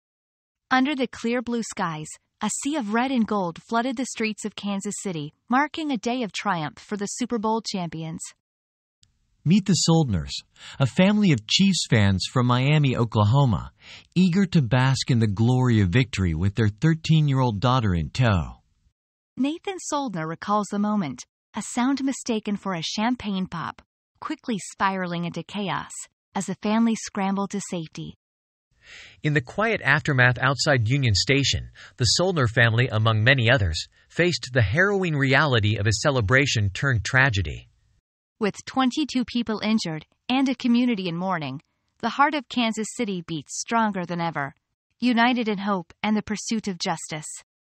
Chaos at Chiefs Parade: A Family's Harrowing Experience
In the wake of celebration, a family's joy turns to terror at the KC Chiefs parade. Dive into their story and the aftermath of this tragic event.